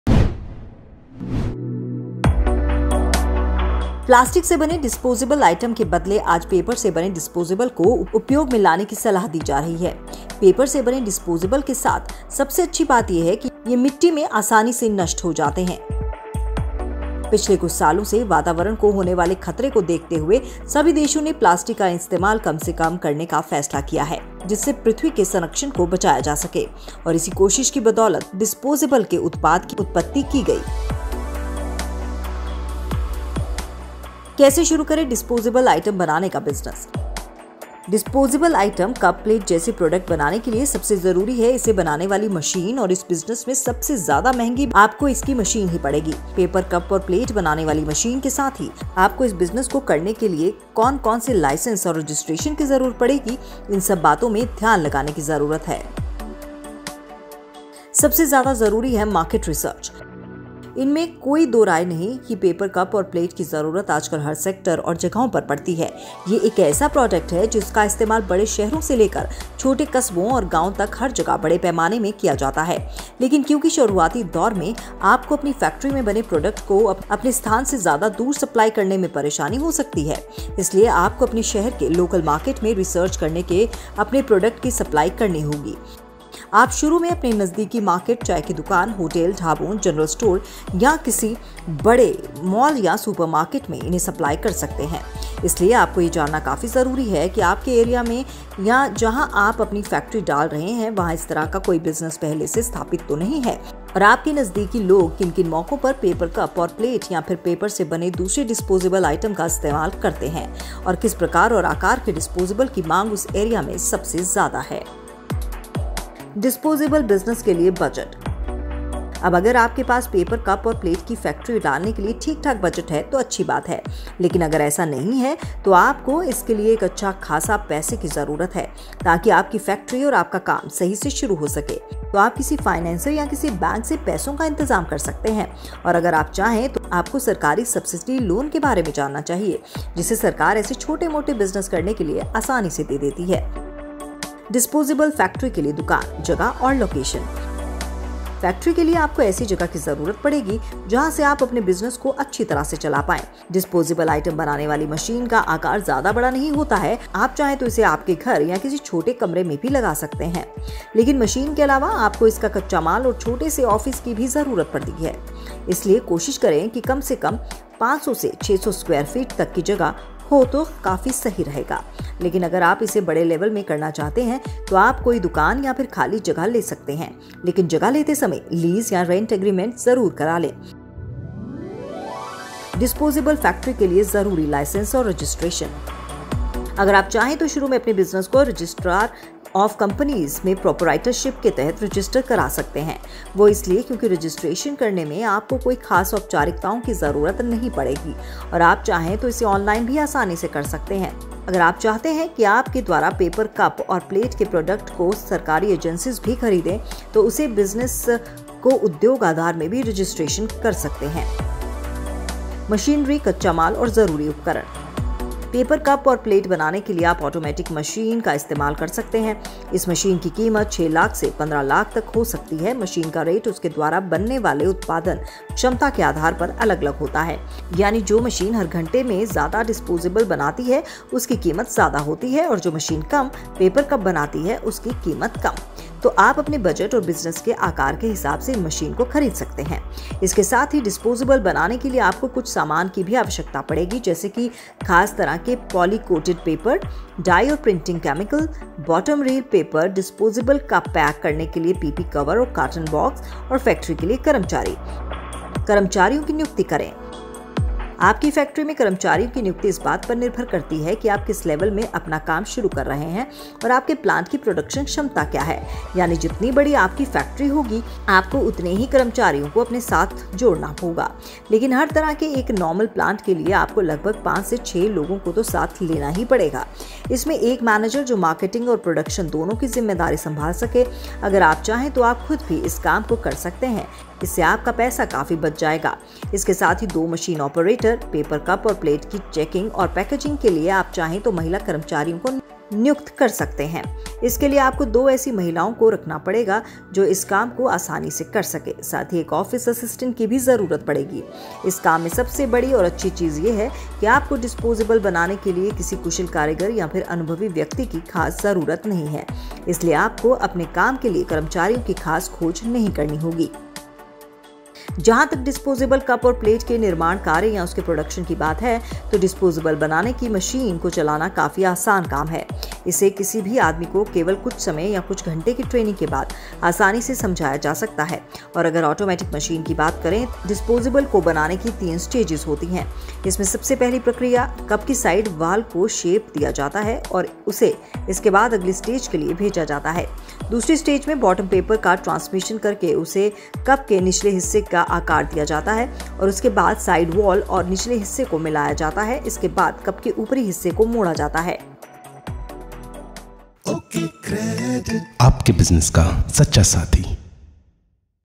घर में ही शुरू करें डिस्पोजेबल बनाने का काम, मिलेगा डबल फायदा | Disposable items Business | OkCredit
प्लास्टिक से बने डिस्पोजेबल आइटम के बदले आज पेपर से बने डिस्पोजेबल को उपयोग में लाने की सलाह दी जा रही है। पेपर से बने डिस्पोजेबल के साथ सबसे अच्छी बात ये है की ये मिट्टी में आसानी से नष्ट हो जाते हैं। पिछले कुछ सालों में वातावरण को होने वाले खतरे को देखते हुए सभी देशों ने प्लास्टिक का इस्तेमाल कम से कम करने का फैसला किया था जिससे पृथ्वी के संरक्षण को बचाया जा सके। और इसी कोशिशों की बदौलत पेपर से बने डिपोजेबलों के उत्पाद की उत्पति की गई। पेपर से बने डिस्पोजेबल की दूसरी सामग्री से बने डिस्पोजेबल की तुलना में कई फायदे हैं क्योंकि इन्हें देखने में स्टाइलिश, सुंदर और जल्दी नष्ट हो जाने के लिए बनाया है। इसलिए दुनियाभर में इसका काफी इस्तेमाल किया जाता है। डिस्पोजेबल आइटम को बड़ी ही आसान प्रक्रिया से बनाया जाता है और पेपर से बने डिस्पोजेबल को बनाने में ज्यादा कचरा भी नहीं पैदा होता और इन्हें दोबारा रीसायकल करना भी काफी आसान है। इनका उपयोग सभी तरह के फंक्शन, पार्टी, पिकनिक, शादी, समारोह के साथ ही
रोड साइड चाट, चाय की दुकान, मॉल में मिलने वाले खाने से लेकर कई जगह बड़े पैमाने में किया जाता है।

डिस्पोजेबल कप-प्लेट के लिए बिजनेस प्लान:

जब आपको पता चल जाए की आपके एरिया में पेपर से बने कप-प्लेट या डिस्पोजेबल की मांग काफी ज्यादा है तो आपको अपने बिजनेस के लिए प्लान बनाना होगा जिससे आप उसे तरक्की की राह पर ले जा सके।
बिजनेस प्लान को सफल बिजनेस का रोड मैप कहा जाए तो गलत नहीं होगा. किसी भी बिजनेस के लिए सटीक प्लान काफी जरूरी है.
आप चाहें तो अपना बिजनेस प्लान एक कॉपी या फिर डायरी बना कर उसमें लिख सकते हैं. इसमें ना सिर्फ आपके आज का बल्कि फ्यूचर में आपका बिजनेस किस तरफ जाएगा और अपने टारगेट तक पहुंचने के लिए आपको क्या क्या करना पड़ेगा इन सब बातों का ख्याल रखना पड़ता है.पेपर से बने डिस्पोजेबल बाजार में कई डिजाईन, रंग, बनावट और आकार के बिकते हैं. इसलिए ये दिखने में स्टाइलिश, खूबसूरत और शानदार भी होते हैं और अगर आप डिस्पोजेबल प्रोडक्ट की मैन्युफैक्चरिंग करना चाहते हैं तो आपको इसके बारे में सभी जरूरी चीजों को जानना काफी जरूरी है |